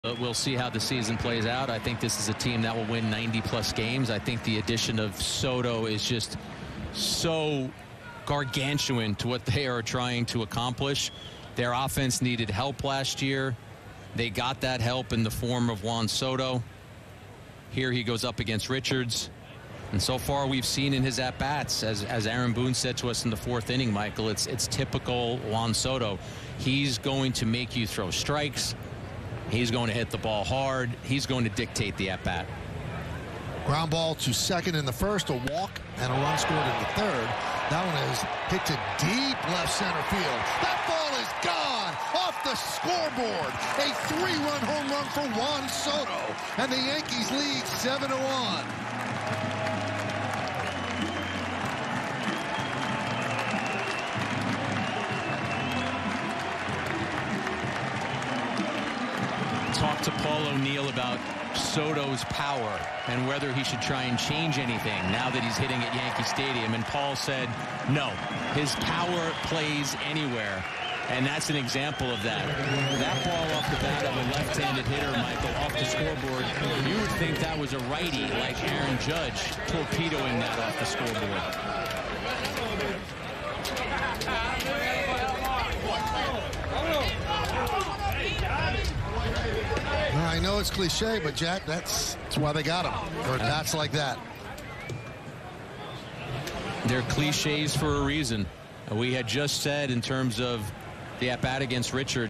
Juan Soto hits his first homer as a Yankee! (Crushed off the scoreboard!)
Juan Soto is officially a Bronx bomber.